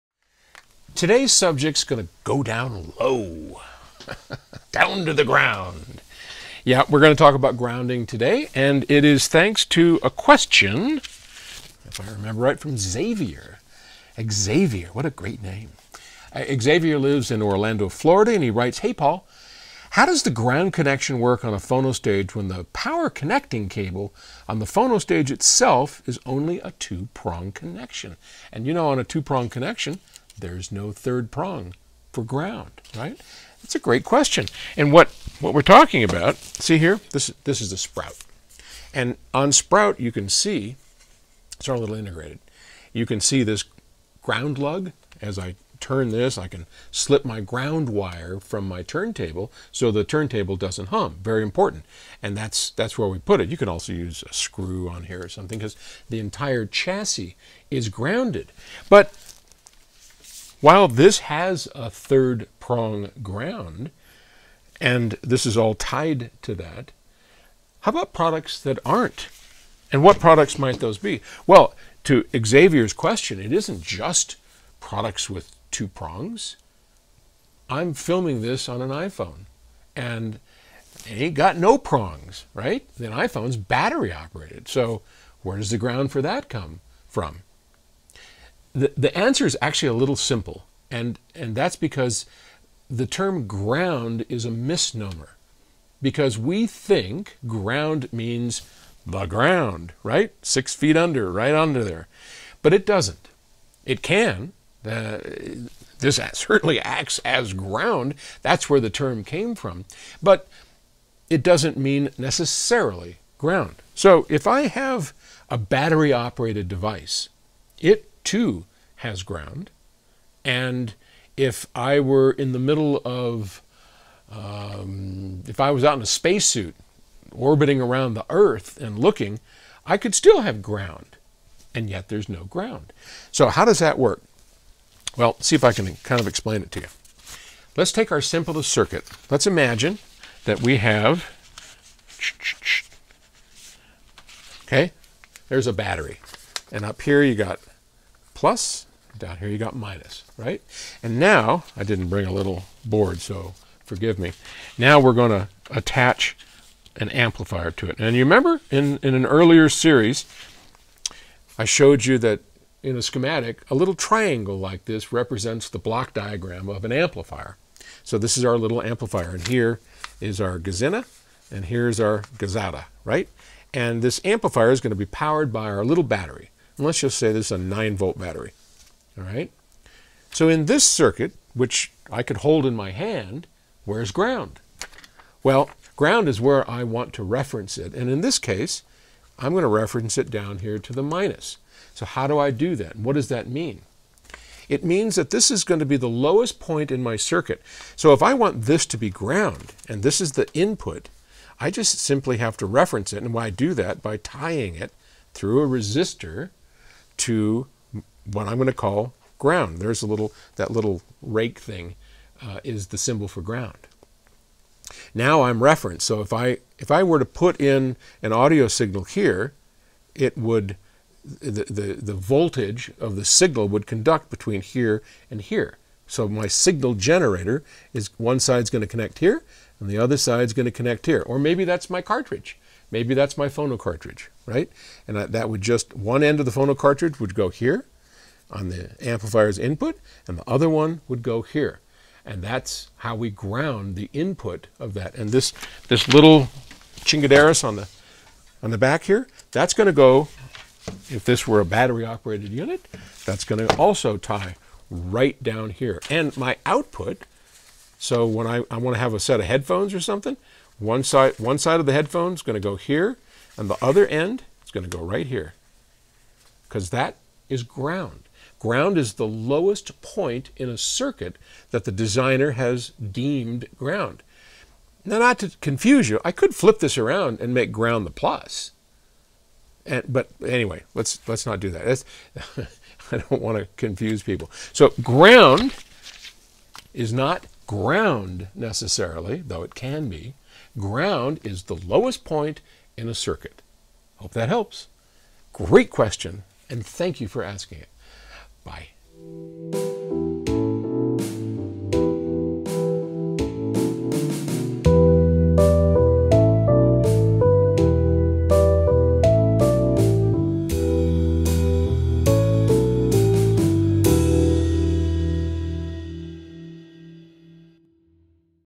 What is signal ground?
How can equipment with only 2-prong plugs have ground? There is no earth ground, right? Paul explains in easy to understand terms all about that mysterious subject, ground. Have a question you'd like Paul to answer for you? and post it.